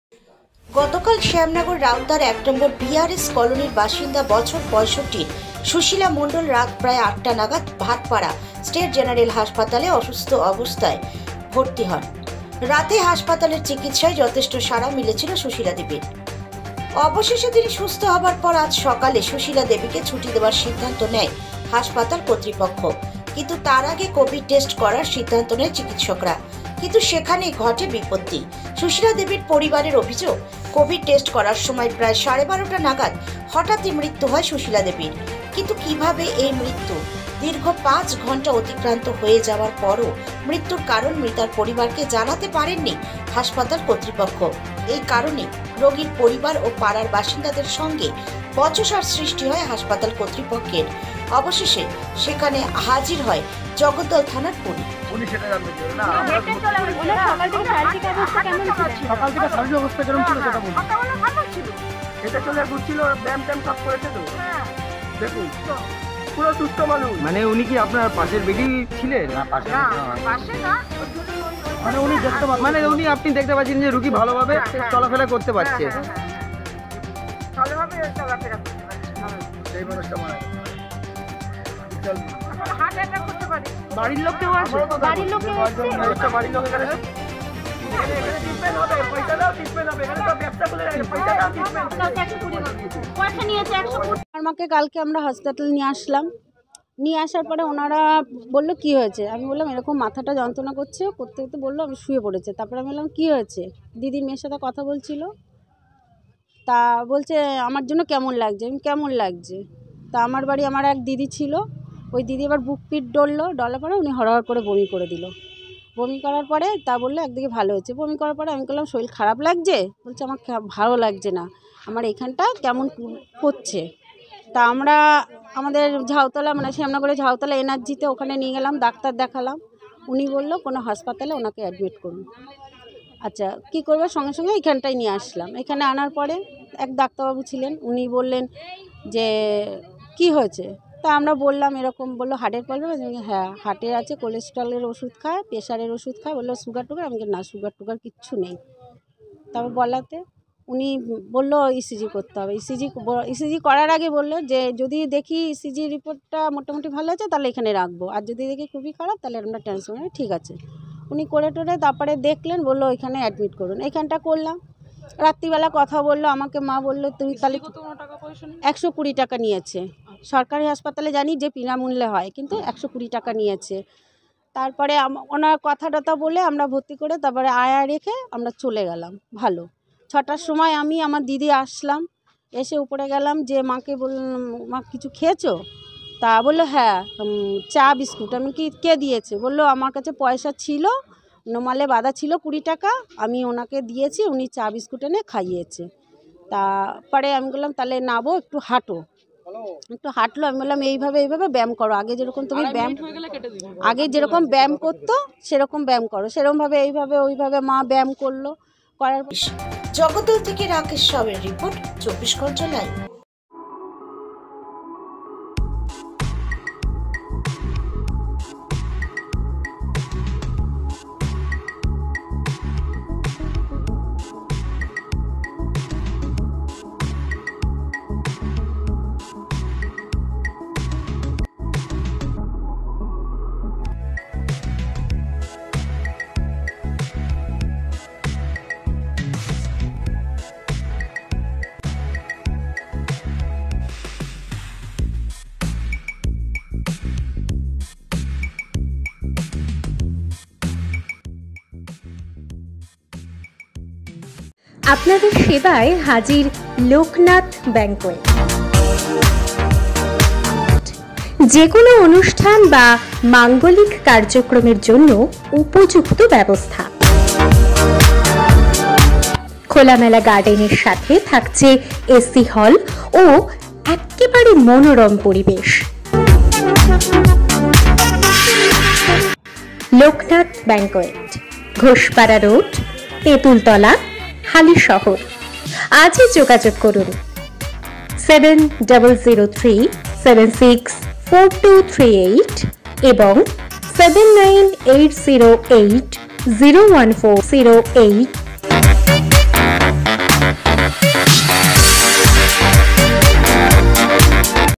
ভাটপাড়া স্টেট জেনারেল হাসপাতালে ঘটলো রোগীর অস্বাভাবিক মৃত্যু
গতকাল শ্যামনগর রাউতার এক নম্বর বিআরএস কলোনির বাসিন্দা বছর ৬৫ সুশীলা মন্ডল রাত প্রায় আটটা নাগাদ ভাটপাড়া স্টেট জেনারেল হাসপাতালে  অসুস্থ অবস্থায় উনার বাড়ির লোক ভর্তি করান। রাতেই হাসপাতালের চিকিৎসায় যথেষ্ট সাড়া মিলেছিলো সুশীলা দেবীর। অবশেষে তিনি সুস্থ হবার পর আজ সকালে  সুশীলা দেবীকে ছুটি দেওয়ার সিদ্ধান্ত নেয় হাসপাতাল কর্তৃপক্ষ। কিন্তু তার আগে কোভিড টেস্ট করার সিদ্ধান্ত নেয় চিকিৎসকরা। কিন্তু সেখানেই ঘটে বিপত্তি। সুশীলা দেবীর পরিবারের অভিযোগ কভিড টেস্ট করার সময় প্রায় 12:30 নাগাদ হঠাৎই মৃত্যু হয় সুশীলা দেবীর। কিন্তু কিভাবে এই মৃত্যু ?  দীর্ঘ 5 ঘন্টা অতিক্রান্ত হয়ে যাওয়ার পরও মৃত্যুর কারণ মৃতার পরিবারকে জানাতে পারেননি হাসপাতাল কর্তৃপক্ষ ।
এই কারণে রোগীর পরিবার ও পাড়ার বাসিন্দা দের সঙ্গে বচসার সৃষ্টি  হয় হাসপাতাল কর্তৃপক্ষের। অবশেষে সেখানে হাজির হয় জগদ্দল থানার পুলিশ।
জগদ্দল থেকে রাকেশ সাউ এর রিপোর্ট 24 ঘন্টা লাইভ